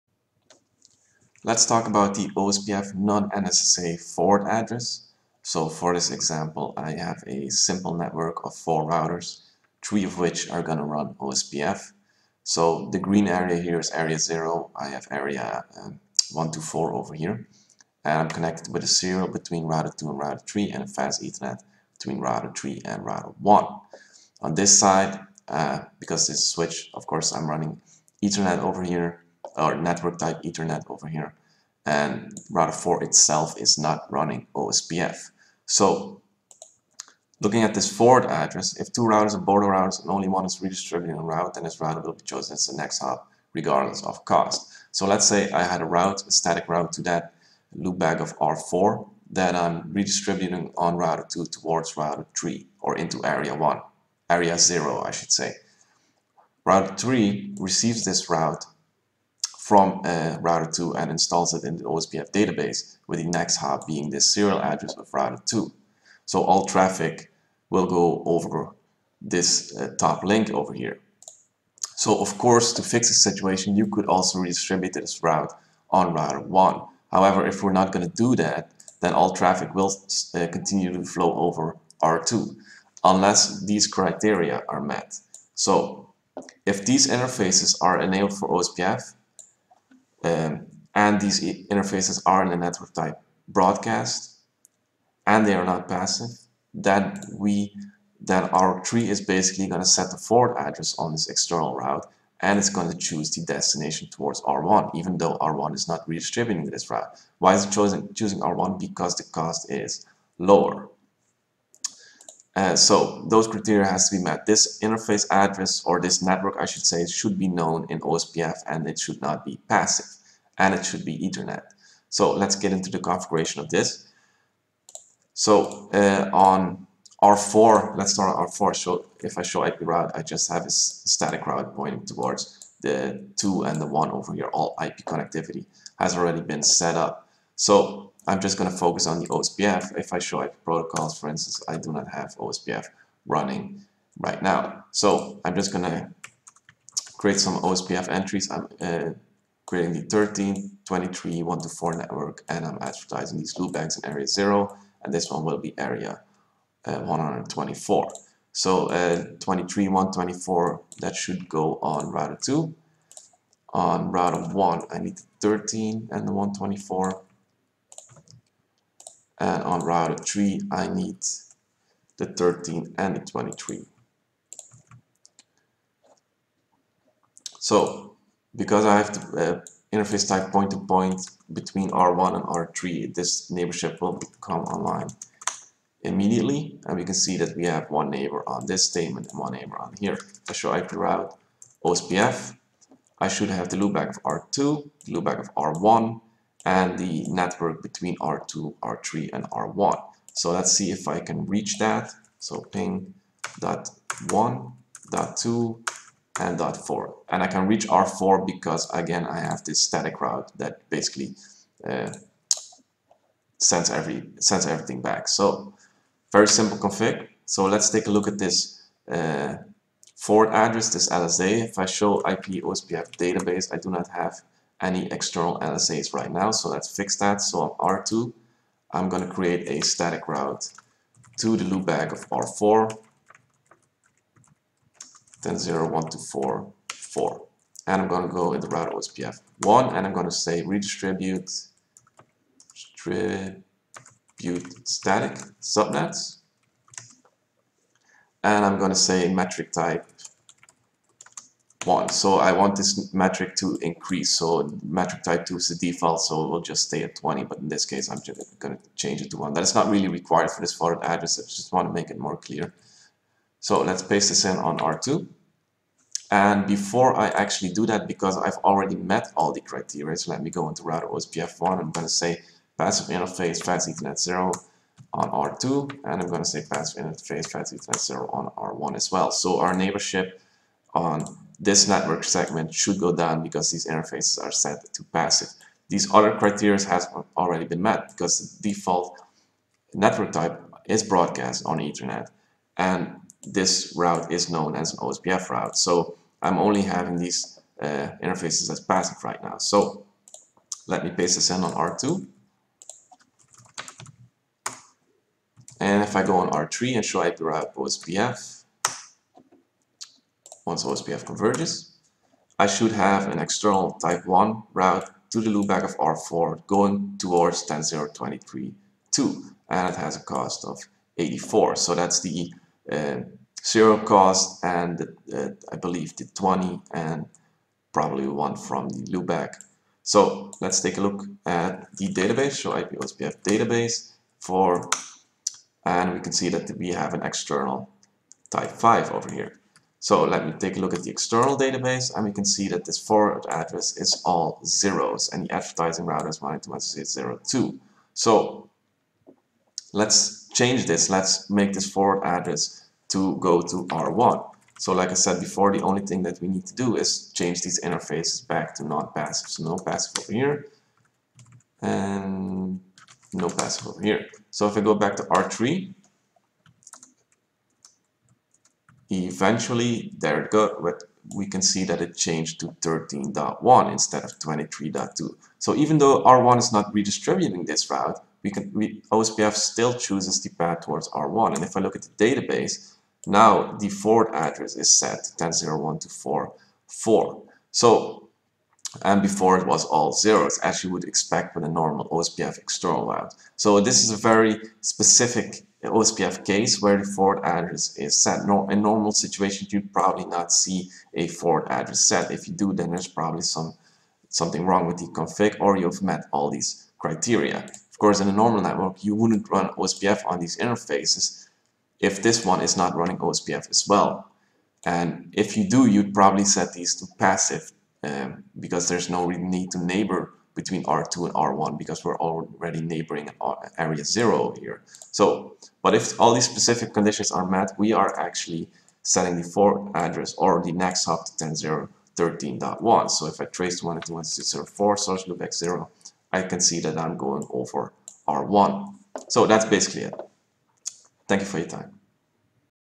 Quick Configs - OSPF Non-NSSA Forward Address (passive, broadcast)
This CCIE oriented episode of quick configs goes into configuring the Non-NSSA Forward Address for OSPF. for all CCIE notes.